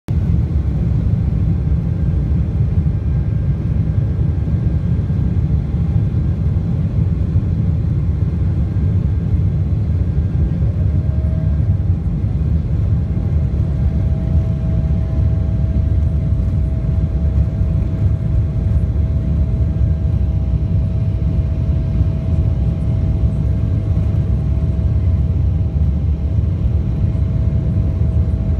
Fly technics of Kuwait Airbus.
27 May at night 3 o’clock start my journey. Dhaka to Kuwait to London Heathrow Airport.